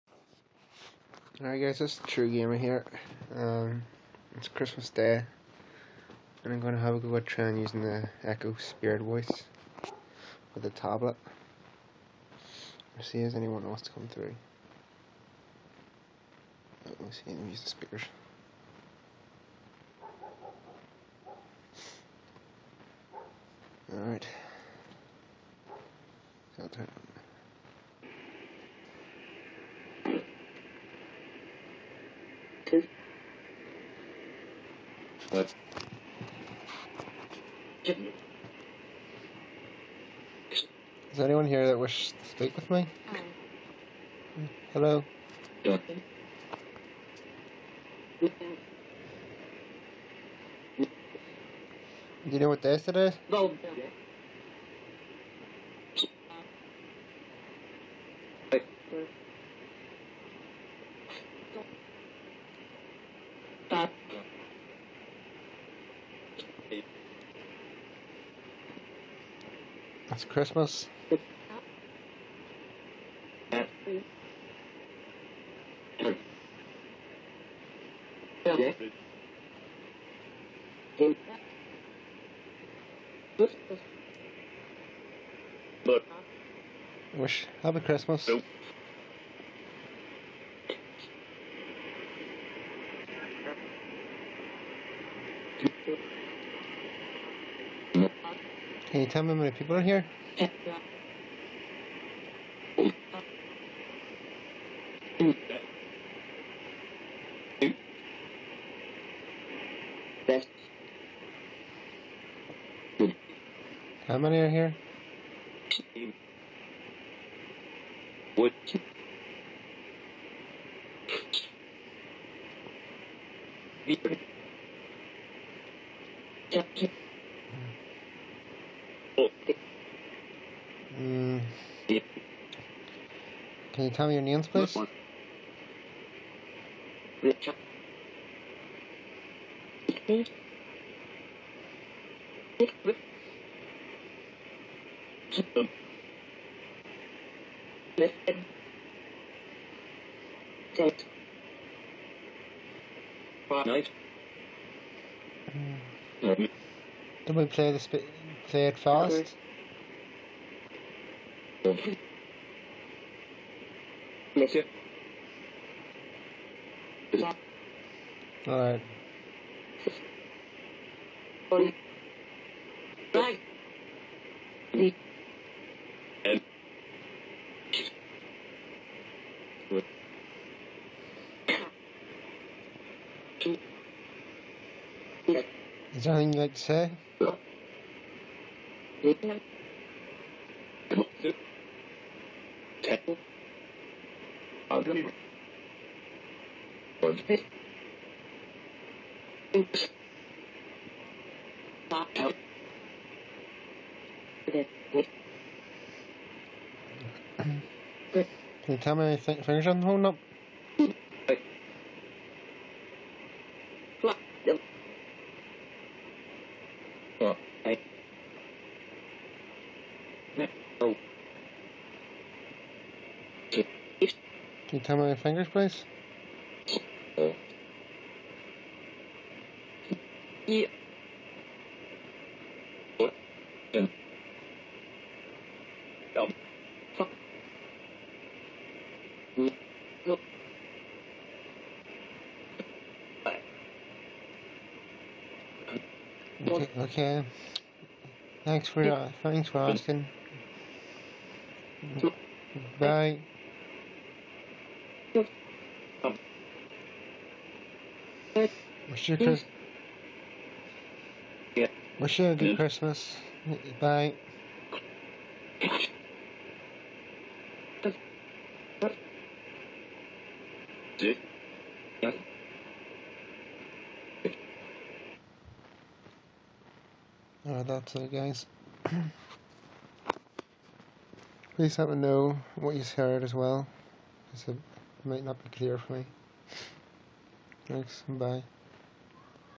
Echo Spirit Voice On Christmas Day
Decided to do the echo spirit voice app on my tablet in my room no bad comments this is my first recording trying it out I just asked a few questions but got really good results with it
please tell me what you here.